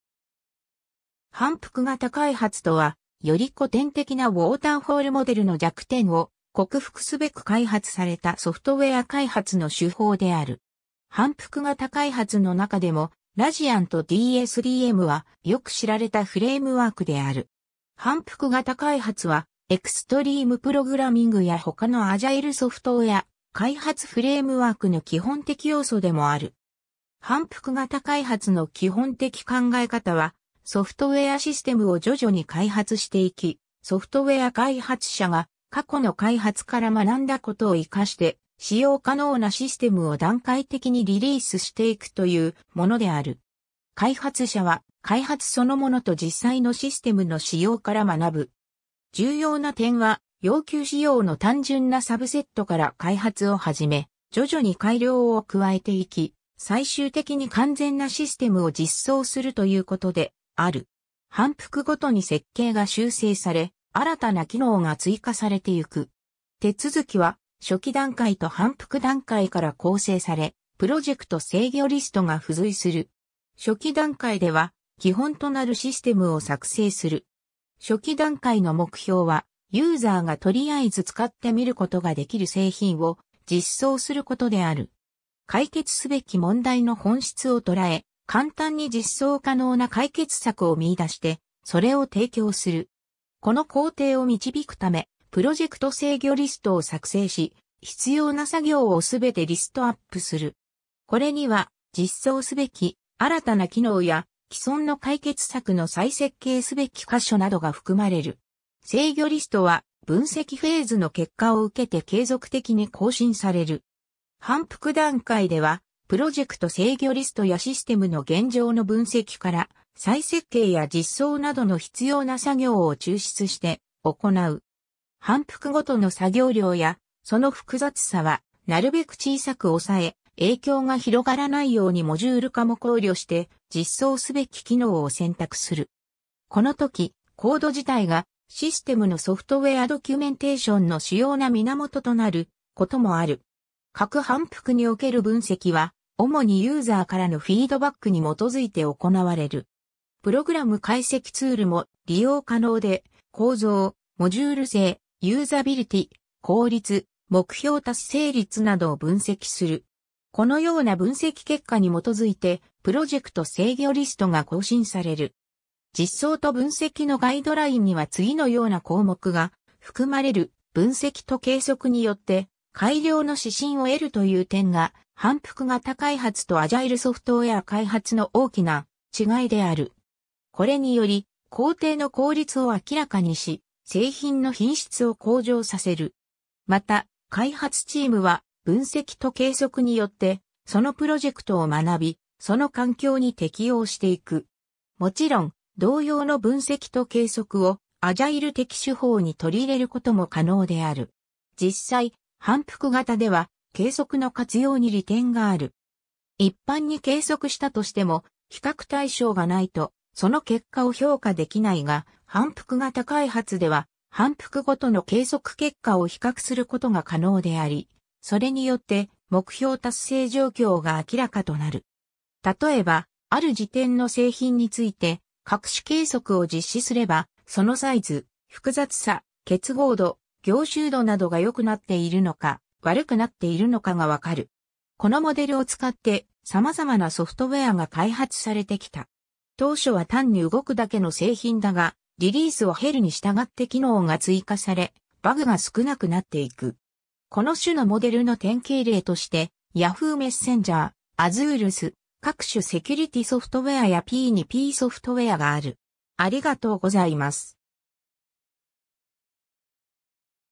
反復型開発
反復型開発（はんぷくがたかいはつ、Iterative and Incremental Development）とは、より古典的なウォーターフォール・モデルの弱点を克服すべく開発されたソフトウェア開発の手法である。
反復型開発の中でもRADとDSDMは、よく知られたフレームワークである。
反復型開発は、エクストリーム・プログラミングや他のアジャイルソフトウェア開発フレームワークの基本的要素でもある。
 反復型開発の基本的考え方は、ソフトウェアシステムを徐々に開発していき、ソフトウェア開発者が過去の開発から学んだことを生かして、使用可能なシステムを段階的にリリースしていくというものである。
開発者は、開発そのものと実際のシステムの使用から学ぶ。
重要な点は、要求仕様の単純なサブセットから開発を始め、徐々に改良を加えていき、最終的に完全なシステムを実装するということである。
反復ごとに設計が修正され、新たな機能が追加されていく。
 手続きは、初期段階と反復段階から構成され、プロジェクト制御リストが付随する。
初期段階では、基本となるシステムを作成する。
初期段階の目標は、ユーザーがとりあえず使ってみることができる製品を実装することである。
解決すべき問題の本質を捉え、簡単に実装可能な解決策を見出して、それを提供する。
この工程を導くため、プロジェクト制御リストを作成し、必要な作業を全てリストアップする。
これには、実装すべき新たな機能や、既存の解決策の再設計すべき箇所などが含まれる。
制御リストは、分析フェーズの結果を受けて継続的に更新される。
 反復段階では、プロジェクト制御リストやシステムの現状の分析から再設計や実装などの必要な作業を抽出して行う。
反復ごとの作業量やその複雑さはなるべく小さく抑え、影響が広がらないようにモジュール化も考慮して、実装すべき機能を選択する。
このとき、コード自体がシステムのソフトウェアドキュメンテーションの主要な源となることもある。
各反復における分析は、主にユーザーからのフィードバックに基づいて行われる。
プログラム解析ツールも利用可能で、構造、モジュール性、ユーザビリティ、効率、目標達成率などを分析する。
このような分析結果に基づいて、プロジェクト制御リストが更新される。
 実装と分析のガイドラインには次のような項目が含まれる: 分析と計測によって改良の指針を得るという点が、反復型開発とアジャイルソフトウェア開発の大きな違いである。
これにより、工程の効率を明らかにし、製品の品質を向上させる。
また、開発チームは、分析と計測によって、そのプロジェクトを学び、その環境に適応していく。
もちろん、同様の分析と計測をアジャイル的手法に取り入れることも可能である。
 実際、反復型では、計測の活用に利点がある。
一般に計測したとしても、比較対象がないとその結果を評価できないが、反復型開発では反復ごとの計測結果を比較することが可能であり、それによって目標達成状況が明らかとなる。
例えば、ある時点の製品について各種計測を実施すれば、そのサイズ/複雑さ/結合度/凝集度などが良くなっているのか悪くなっているのかがわかる。
 このモデルを使って、さまざまなソフトウェアが開発されてきた。
当初は単に動くだけの製品だが、リリースを経るに従って機能が追加され、バグが少なくなっていく。
この種のモデルの典型例として、Yahoo! Messenger、Azureus、各種セキュリティソフトウェアやP2Pソフトウェアがある。